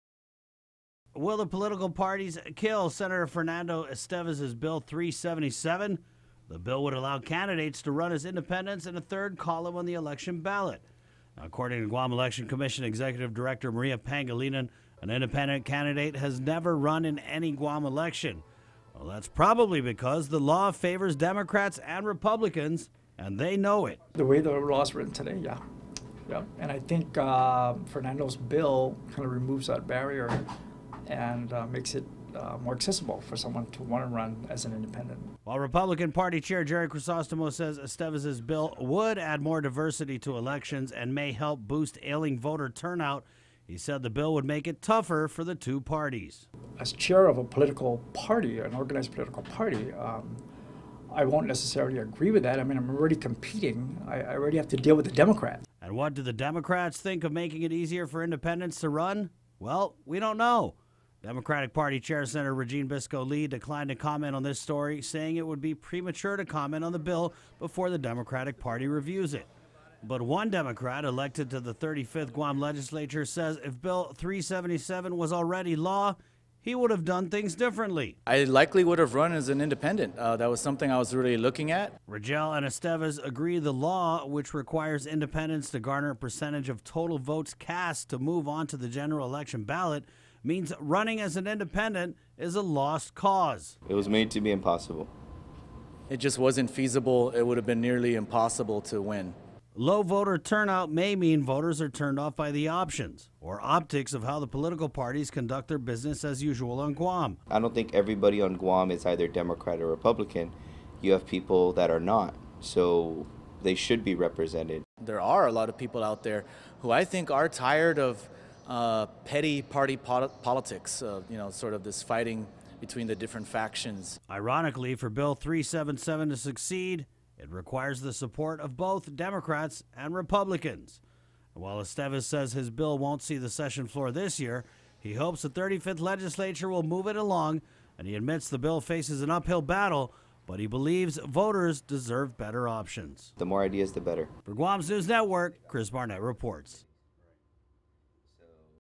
Are independents in our political future?
Will the political parties kill Senator Fernando Esteves' Bill 377 - which would allow candidates to run as Independents in a third column on the election ballot? According to Guam Election Commission Executive Director Maria Pangelinan, an Independent candidate has never run in any Guam election. That's probably because the law favors Democrats and Republicans - and they know it.